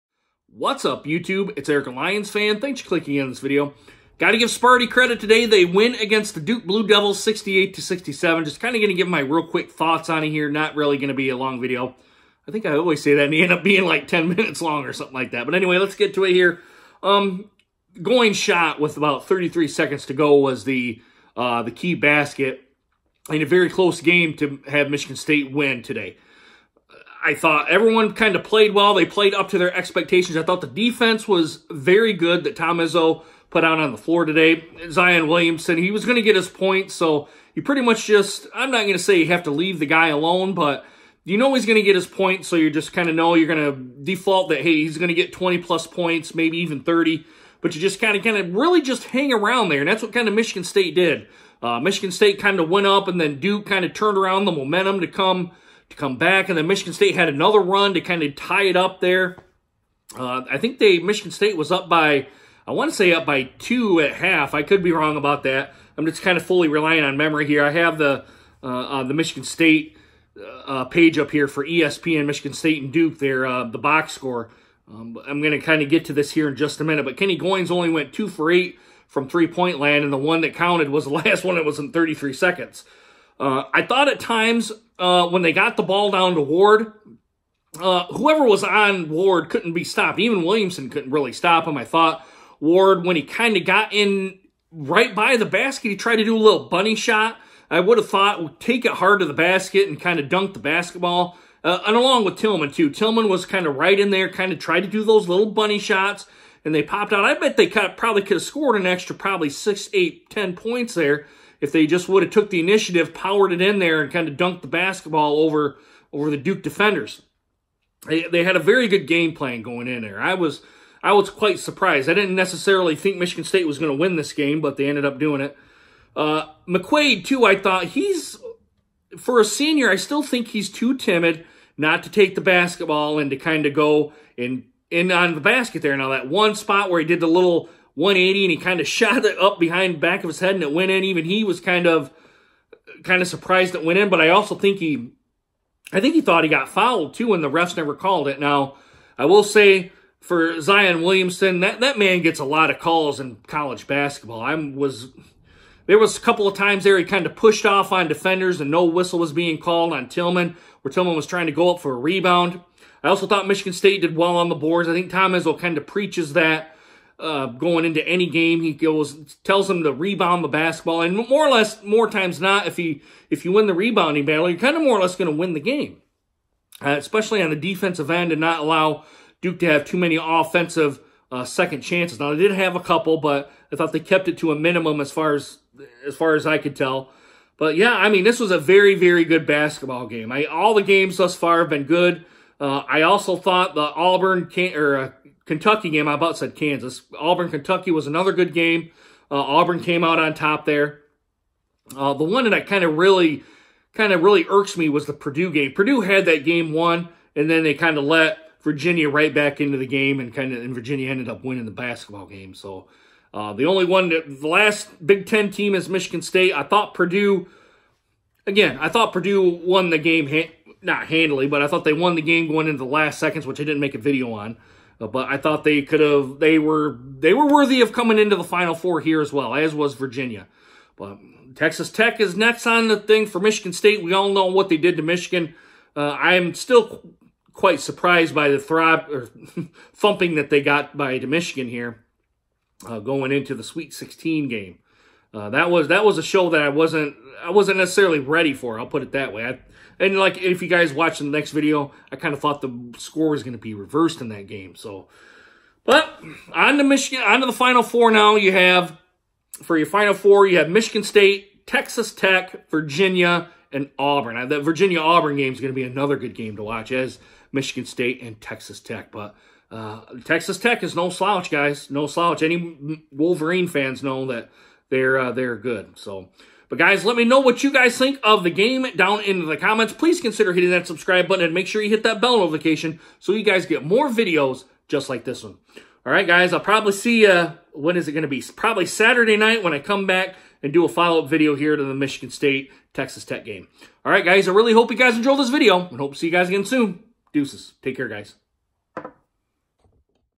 Duke vs Michigan State game reaction! Sparty is on to the Final Four!
Photo Thumbnail credit: Jonathan Newton - The Washington Post

See how well I did in my my NCAA Bracket. -